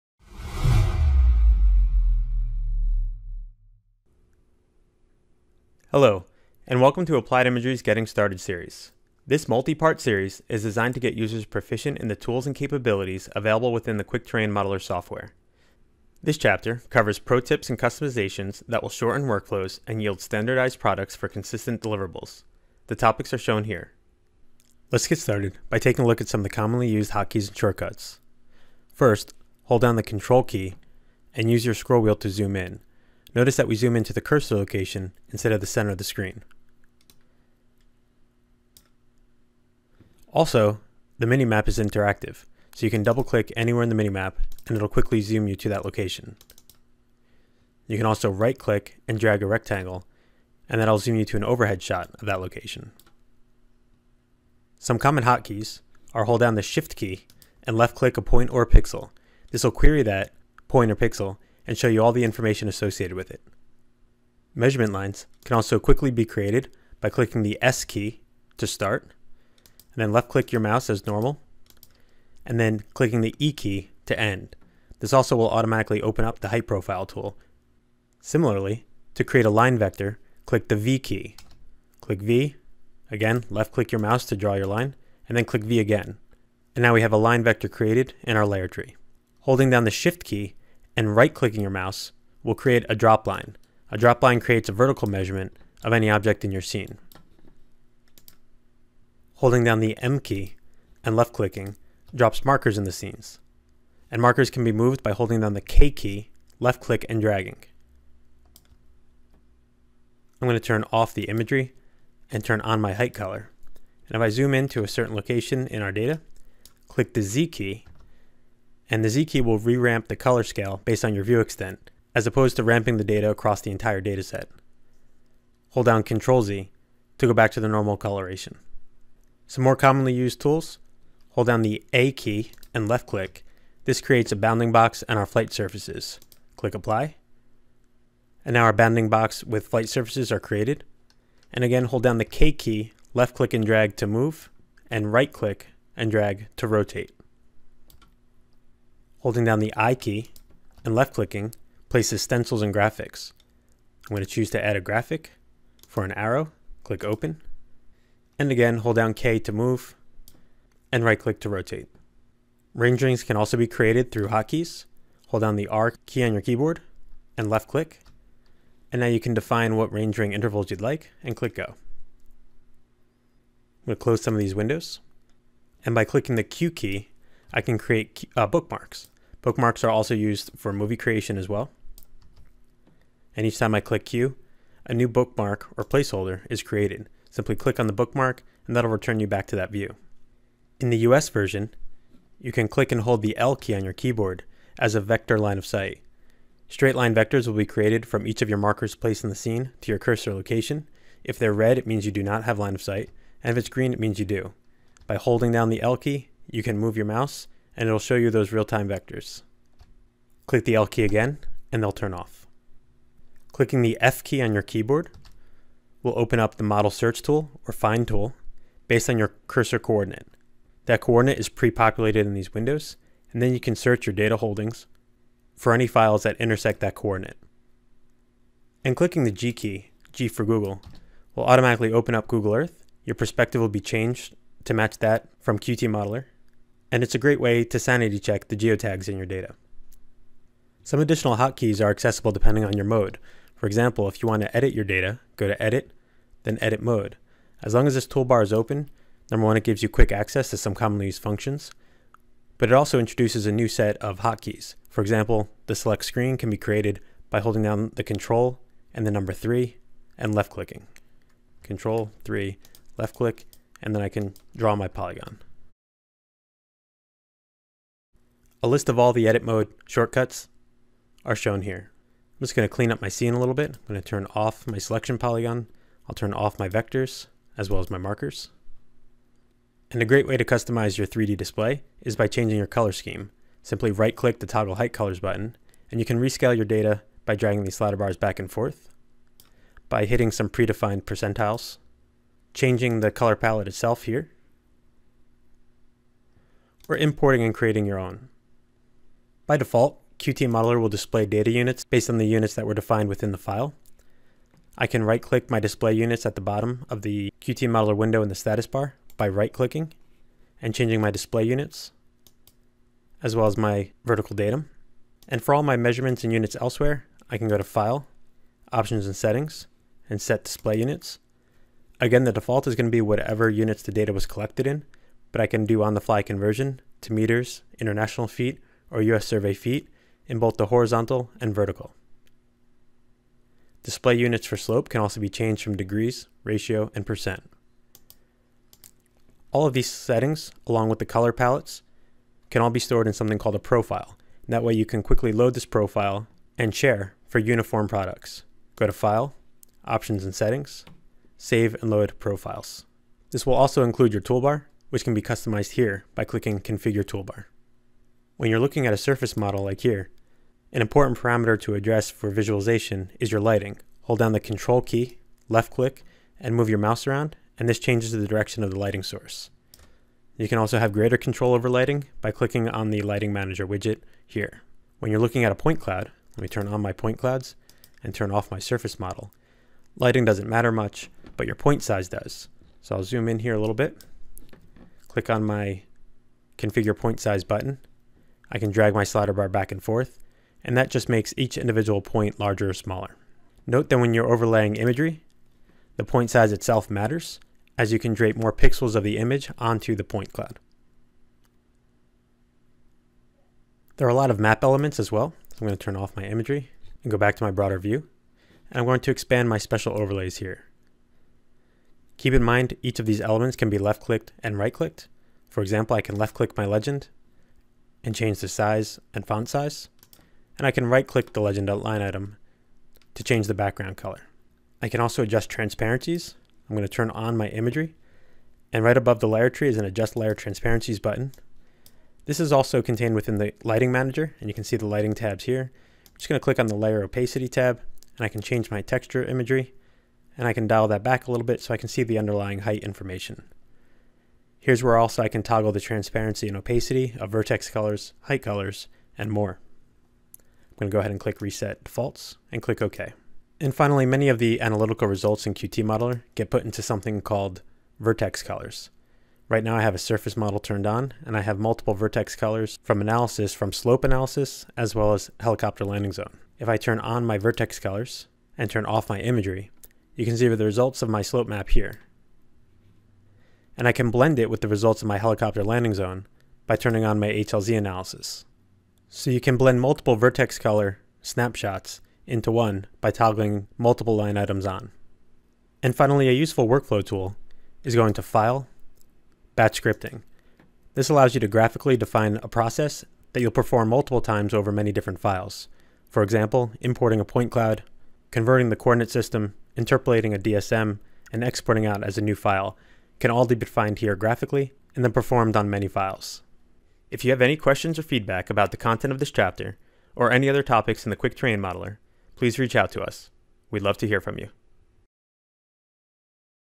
Quick Terrain Modeler v8.4+ Getting Started (8 of 10): Pro Tips and Customization
Topics:
- Hotkeys and Shortcuts
- Edit Toolbar
- Color Palettes
- Display Units and Settings
- Profiles
- Special Overlays
- Transparencies
- Vertex Color Merging